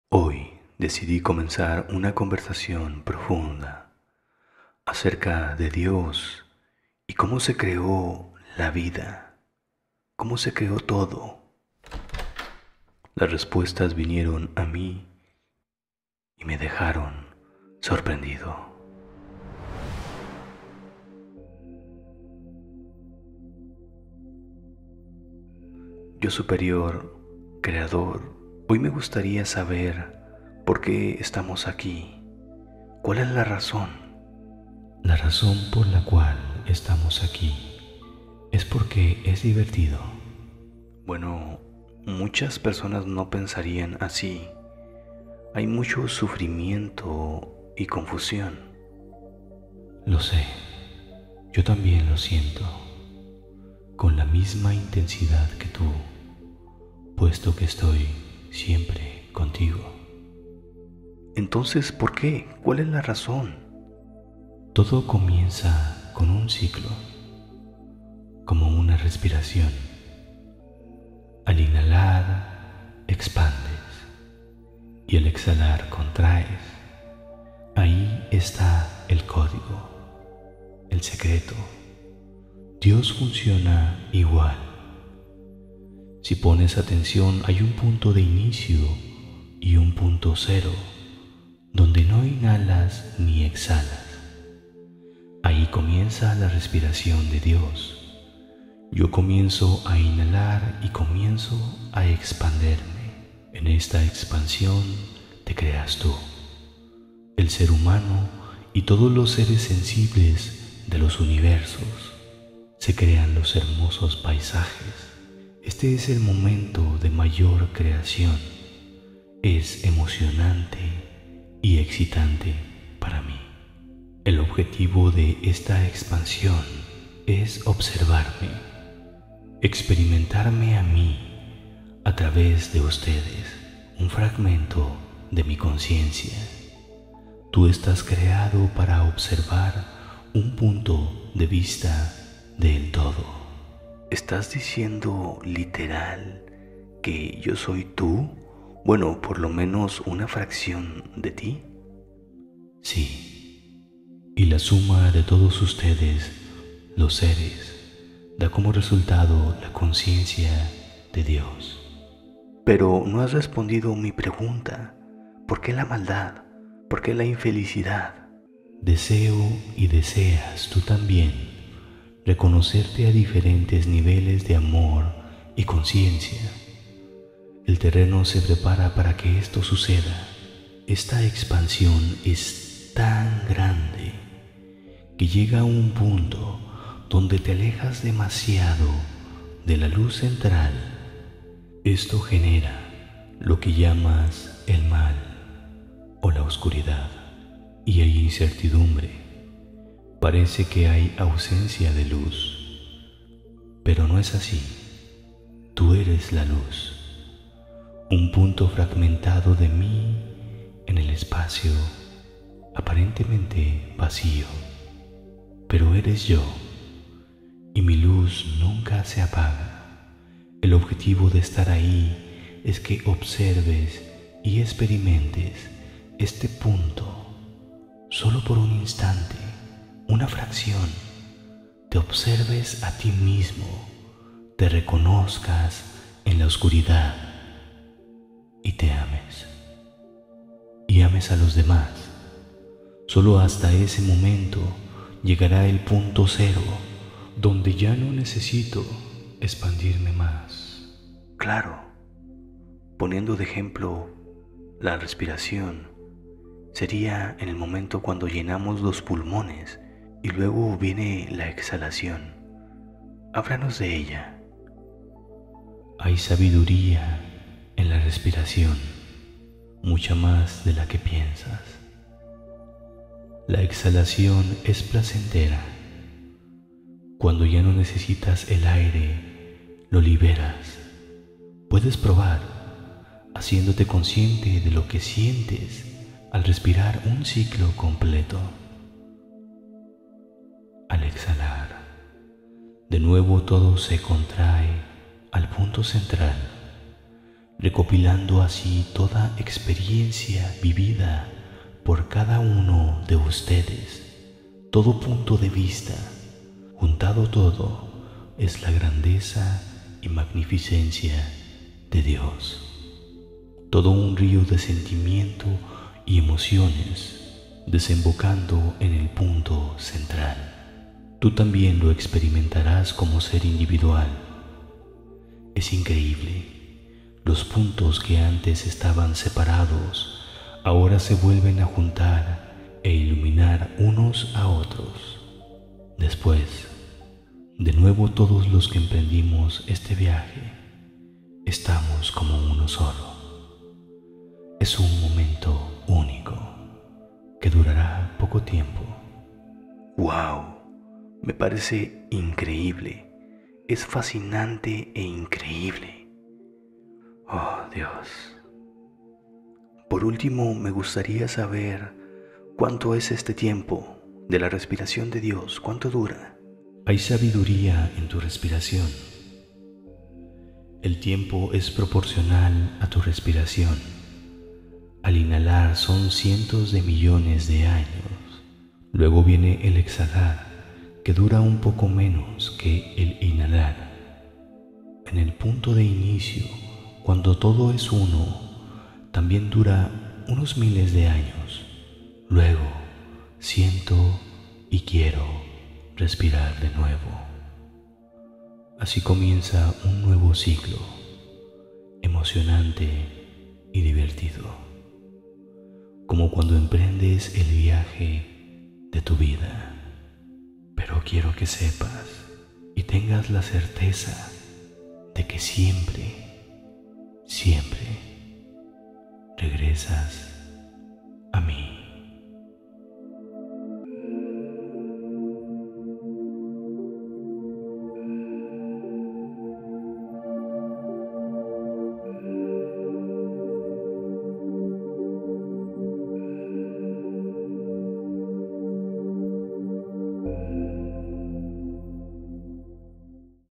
Conversaciones con el "YO SUPERIOR" Capitulo 9
este capitulo es existencial largo y profundo, preguntando sobre la vida y la creación, uno de mis favoritos.